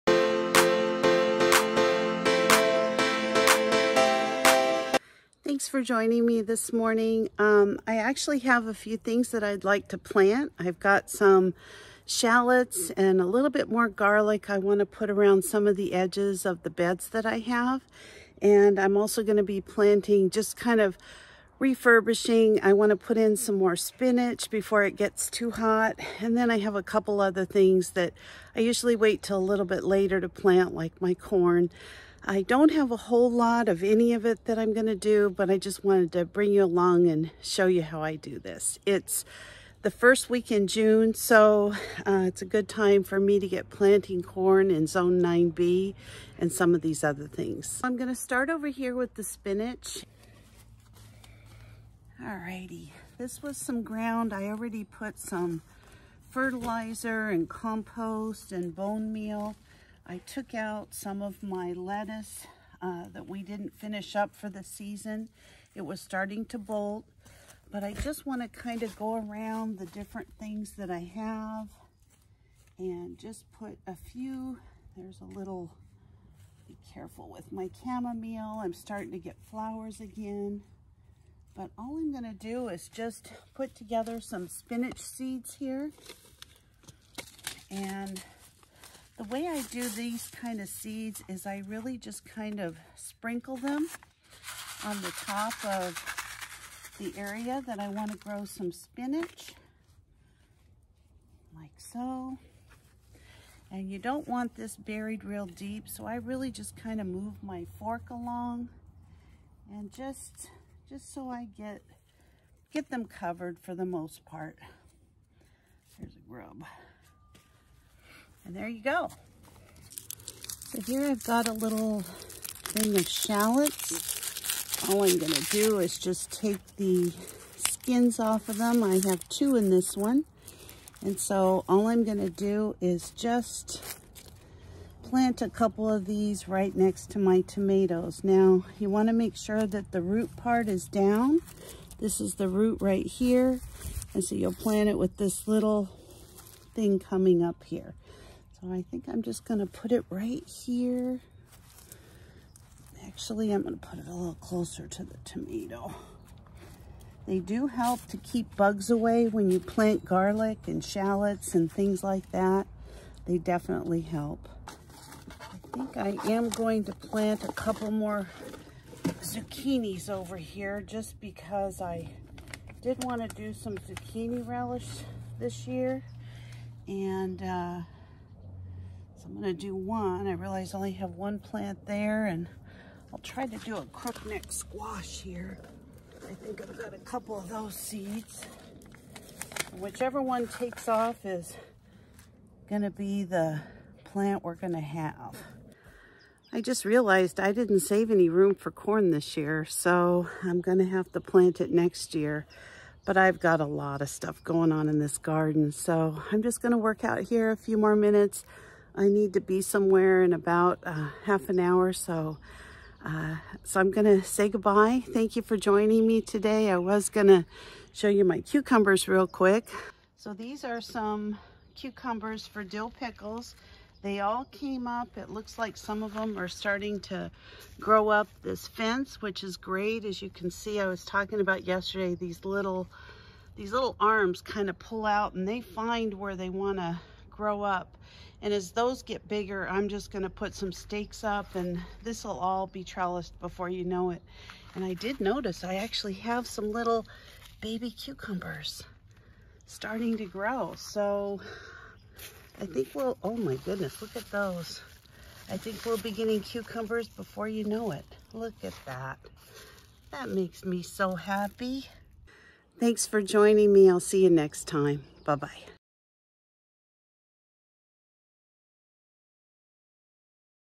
GARDENERS...Whats happening in my GARDEN in JUNE?
This is a quick garden update with some additional seeds I planted this morning.